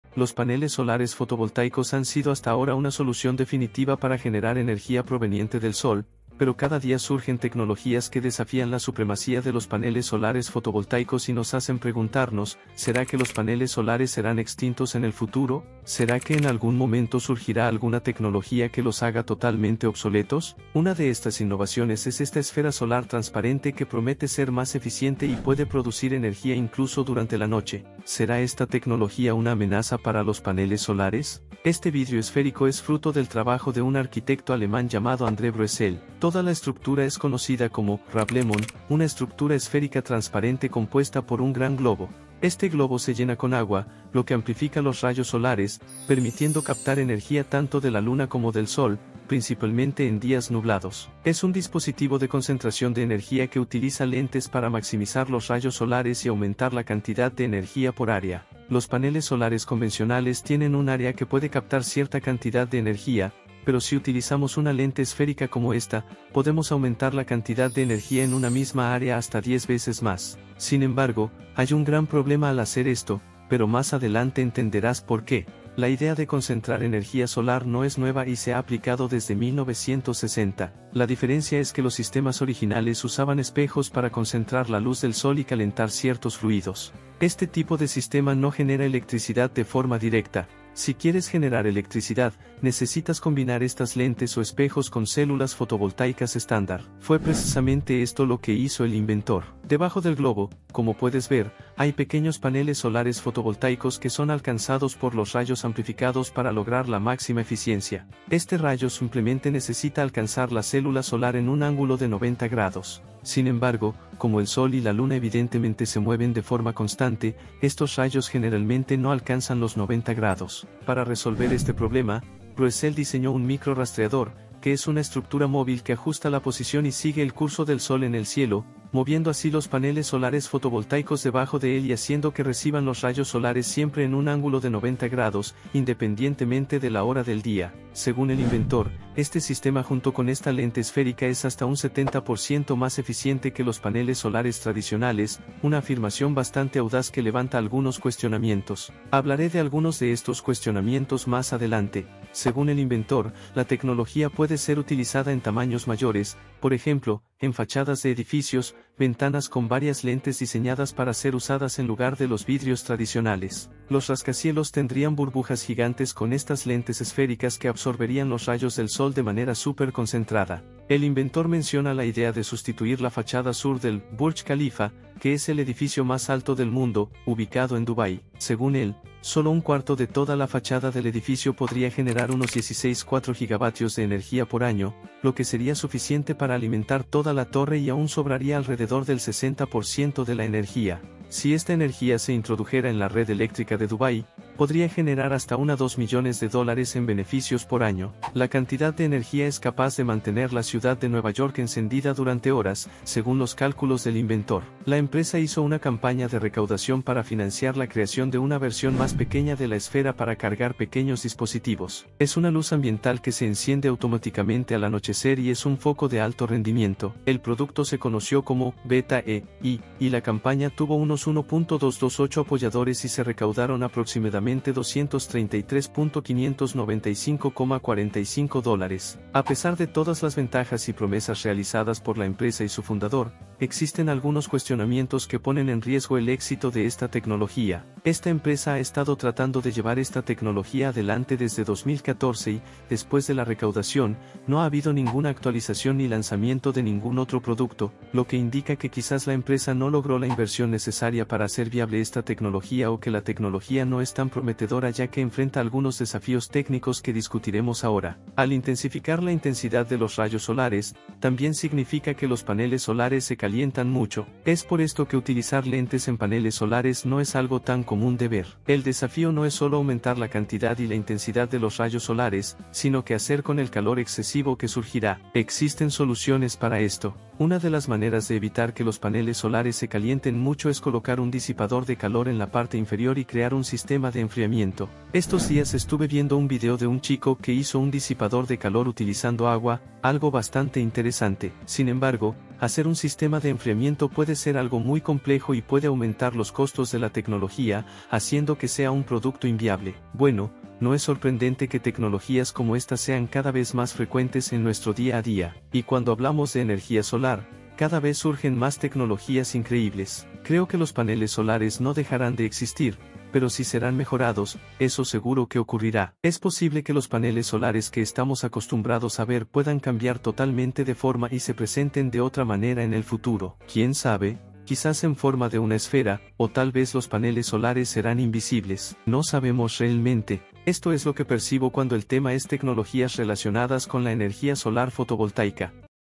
¿ El Fin de los PANELES SOLARES? Bola de Cristal Genera Energía y Promete Ser Más Eficiente
Exploramos una revolución en el mundo de la energía solar: la innovadora estructura Rawlemon. Conocida popularmente como "Bola de Cristal," esta tecnología promete transformar la manera en que captamos y utilizamos la energía solar. A diferencia de los paneles solares tradicionales, Rawlemon es una esfera transparente que puede generar energía tanto de día como de noche, aumentando significativamente la eficiencia energética.